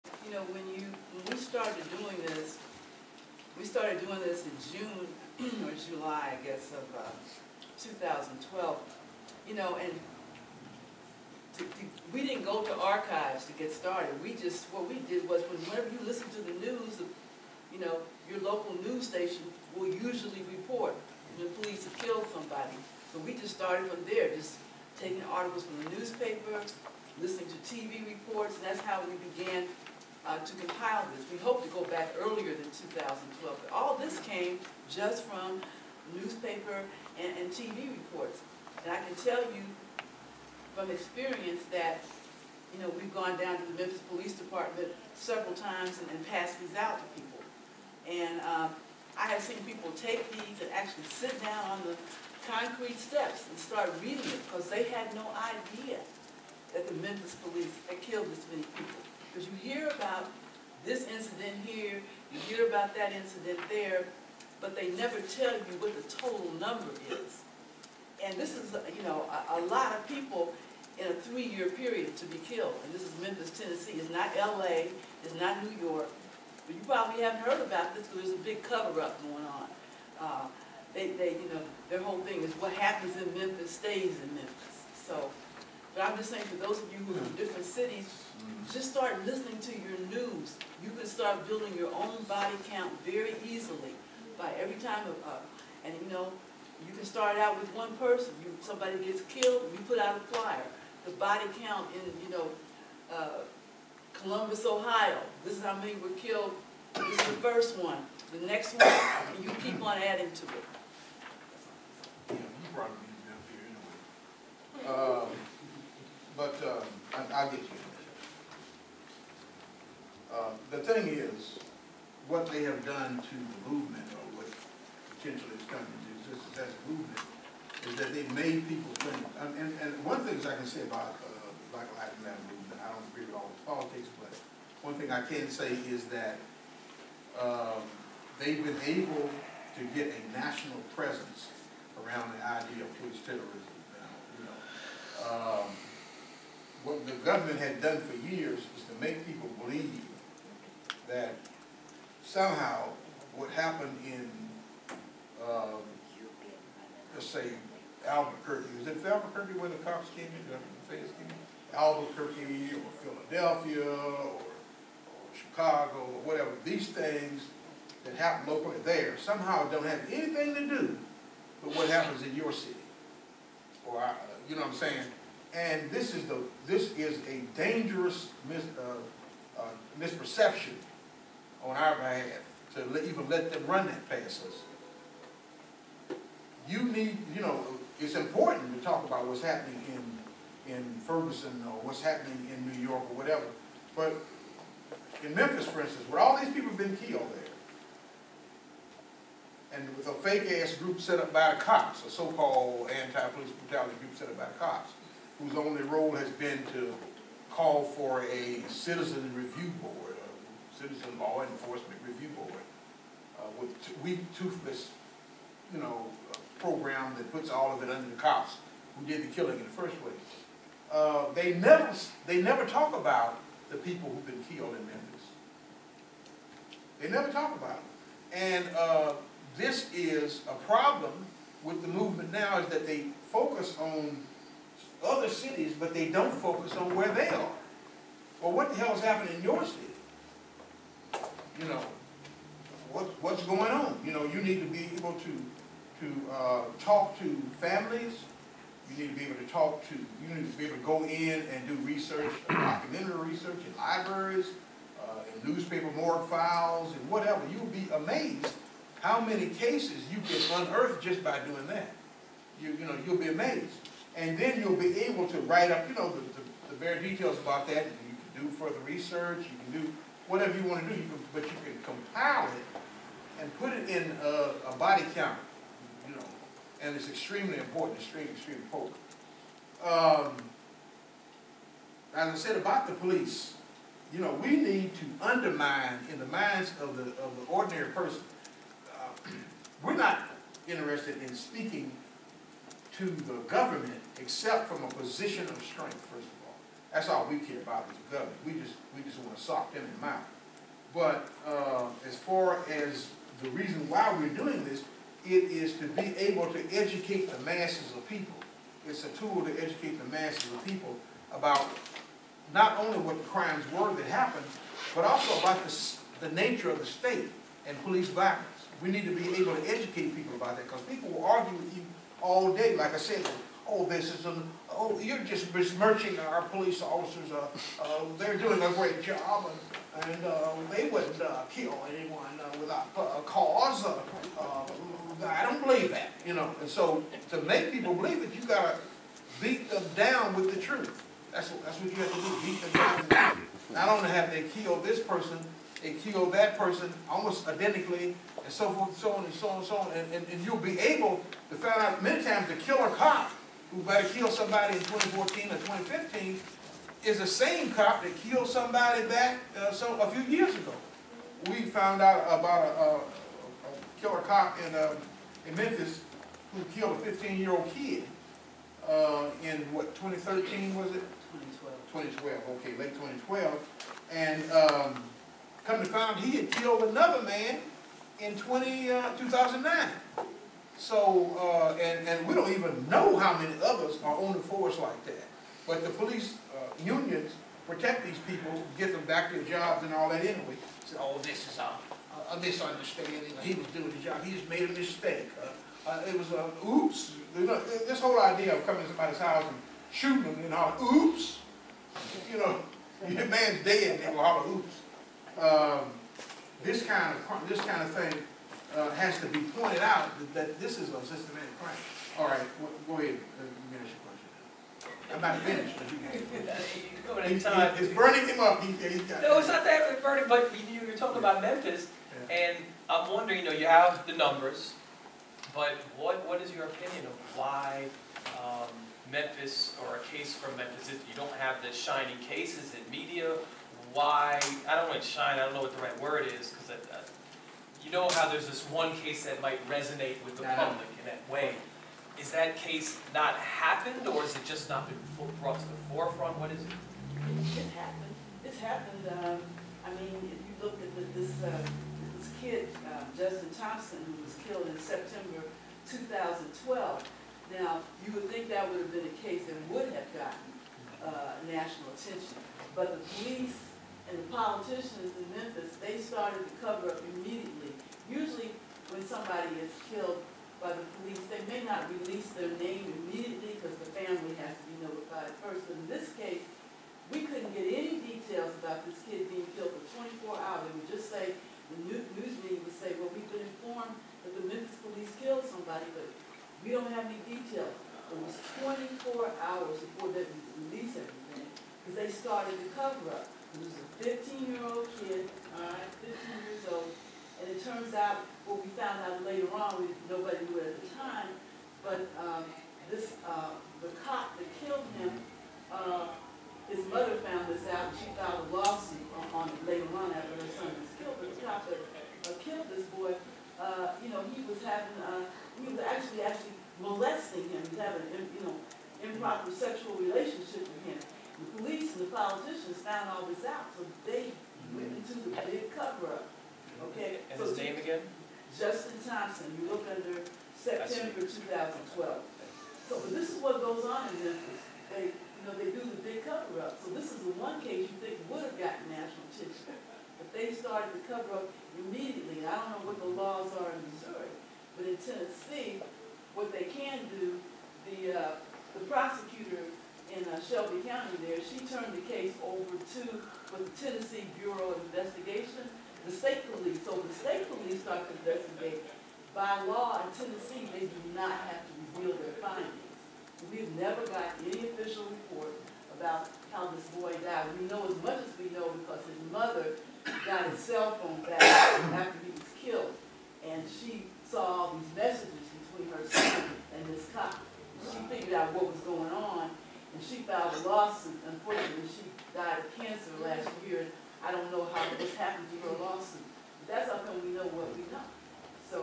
23 Police Killings In 3 Years By Lorenzo Komboa Irvin
Memphis Tennessee has the largest number of police killings in the past three years. People are led to believe that killings in one city has nothing to do with killings in your city

Many thanks to videographer Richard Romanchuk of Acme Video for documenting the 2015 Ida B Wells Conference in Kansas City, Missouri.